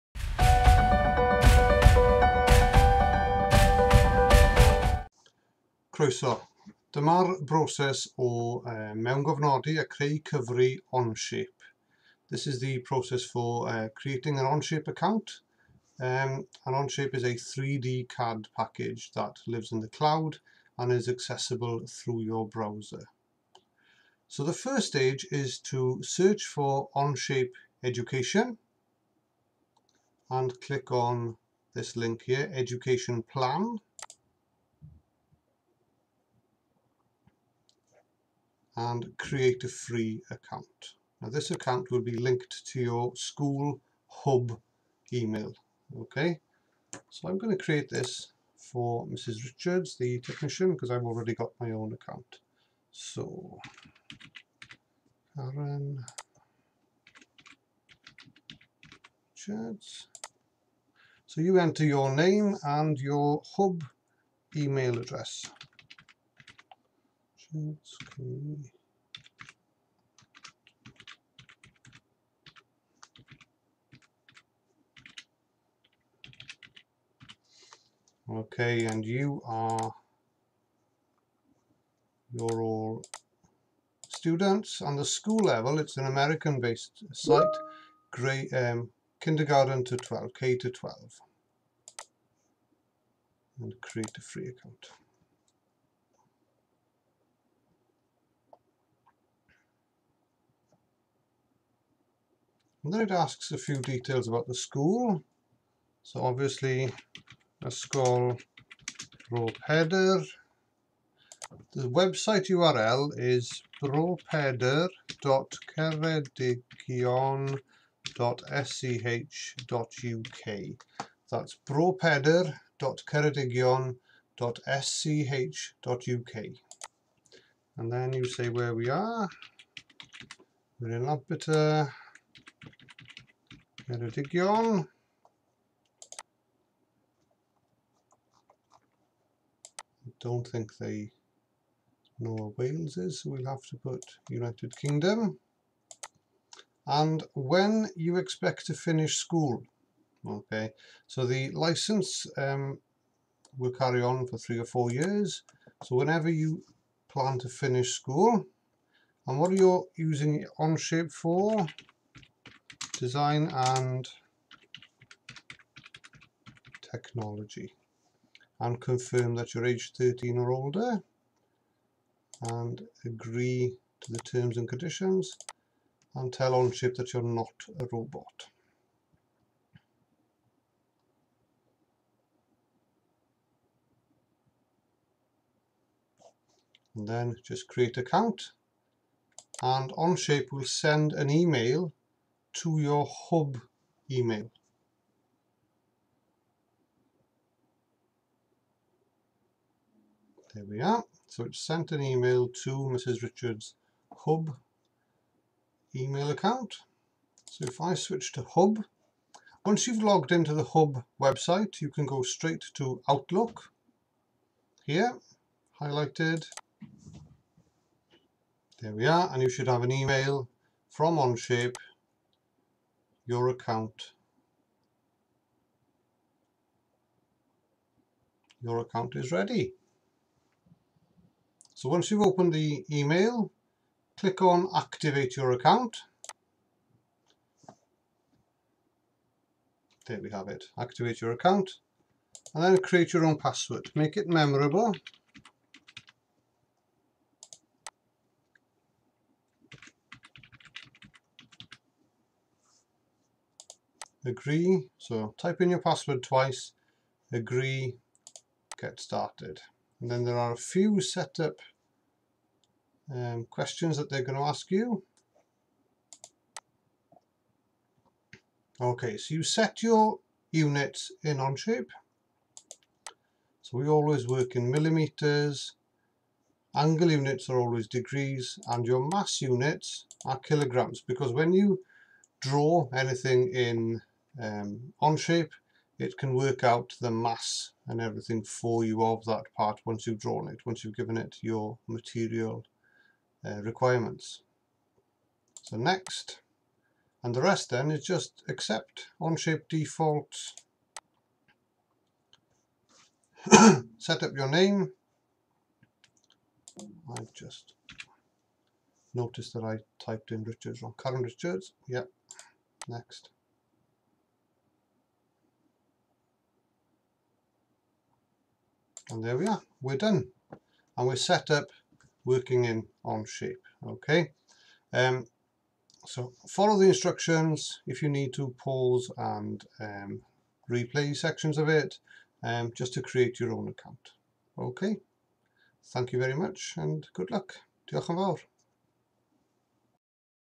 Onshape Account Signup
Details on how to create a free educational account for Onshape 3D CAD.